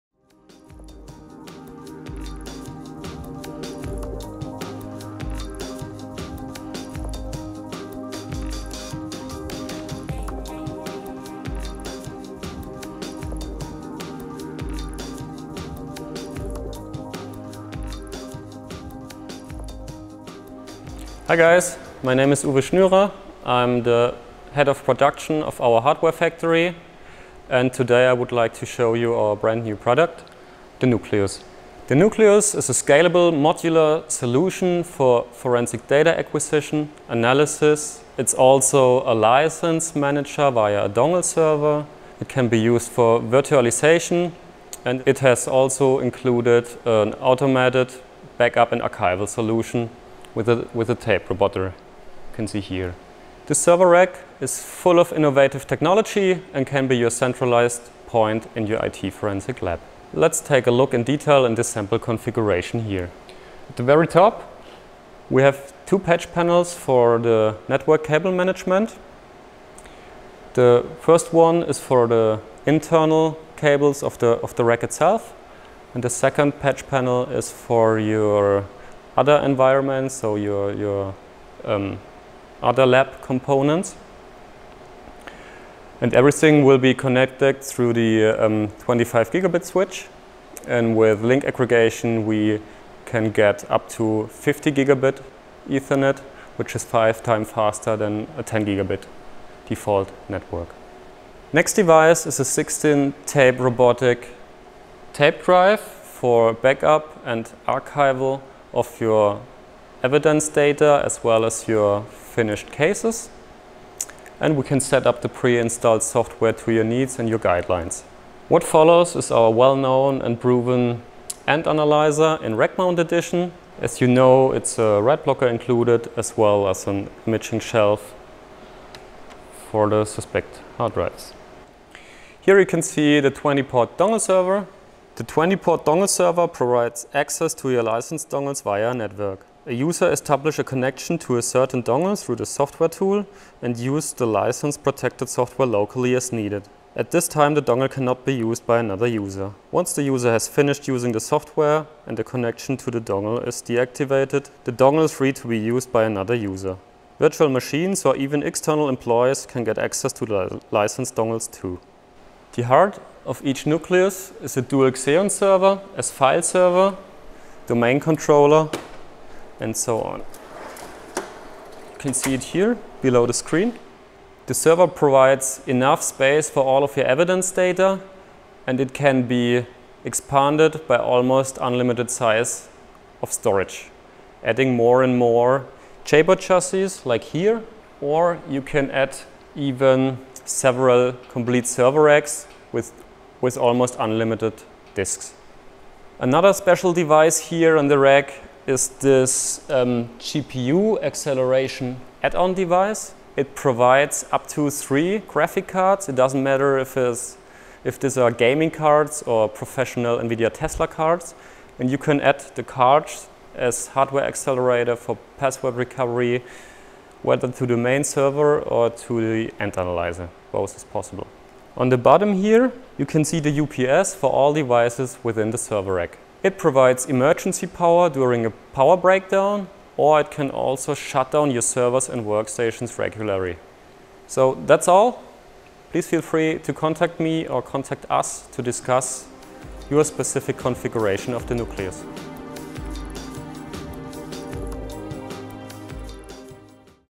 NUCLEUS - the heart of every IT-forensic lab! - by mh SERVICE GmbH
Our most powerful centralised forensics server ever, complete with huge storage capacity!

A scalable solution for data acquisition, analysis, case management, evidence and case data storage. Works effortlessly with all forensics tools.
20 port dongle server for sharing the license dongles within the forensic network, also suitable for optimal use with virtual machines. 2x25Gb network for all devices - Incl. automated tape backup appliance - 25x faster than normal Gigabit Ethernet - Optional complete virtual environment

The versatile applications of the Nucleus:

Nucleus: Centralised server for your IT-forensic laboratory
Optional Add on: Nucelus Security: Remote analytics / endpoint security solution for incidence response
Optional Add on: Nucleus Remote: Centralised server for your IT-forensic laboratory and complete solution for remote access and work in the home office.

- MADE IN GERMANY POWERFUL, TRUSTED & EXTREMELY FAST COMPUTER FORENSIC mh SERVICE GmbH